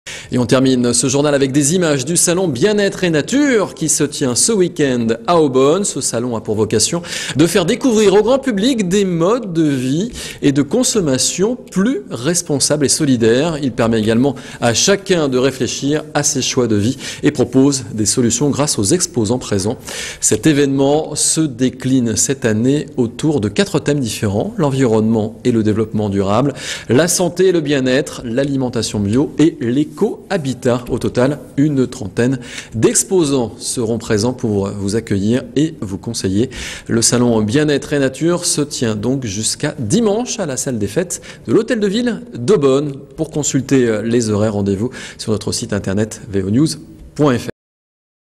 Salon Bien-être et Nature (Eaubonne)
Le salon bien-être et Nature se tient ce week-end à Eaubonne, dans le Val d'Oise. (Droits réservés. Pour toute exploitation commerciale, veuillez nous contacter